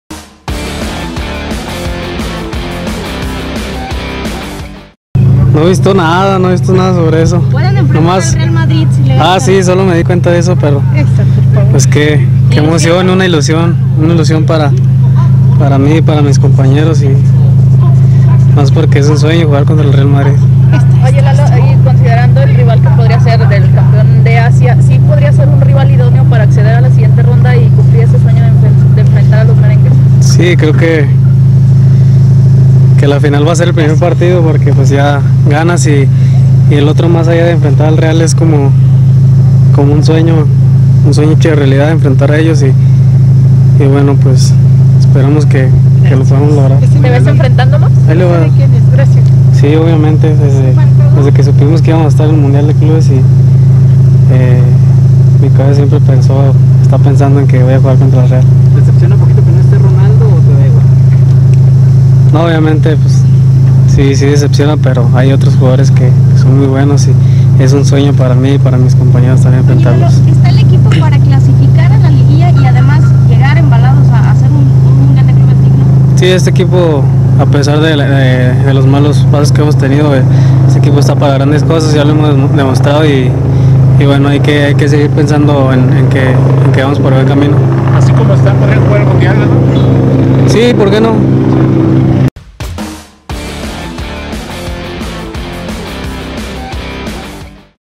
Chivas | "Sería un sueño jugar contra el Real Madrid", López | ONCE Diario
Javier Eduardo López, mediocampista de Chivas, confiesa su entusiasmo por la posibilidad de enfrentar al Real Madrid en el próximo Mundial de Clubes. El reporte de Natalia León.